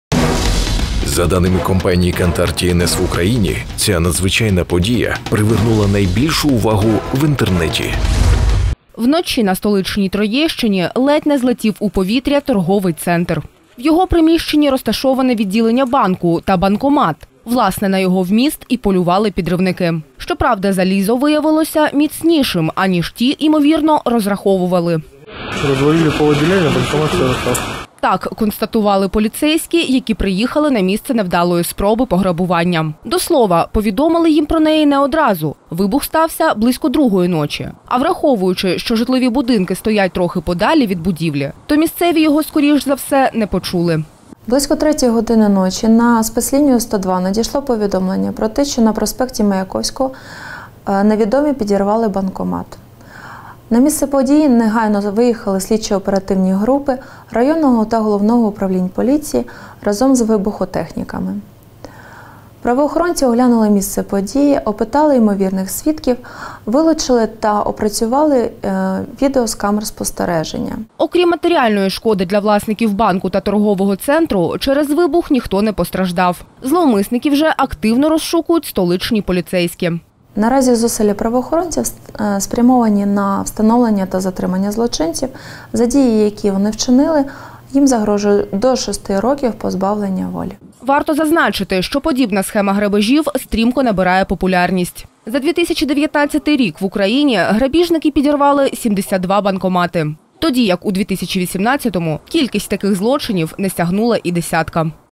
ТОП НОВИНА. На столичній Троєщині зловмисники підірвали відділення банку
Вночі ледь не злетів у повітря торговий центр у Деснянському районі Києва. В його приміщенні розташоване відділення банку та банкомат. Власне, на його вміст і полювали підривники. Щоправда, залізо виявилося міцнішим, аніж ті, імовірно, розраховували. Гроші грабіжникам поцупити не вдалося.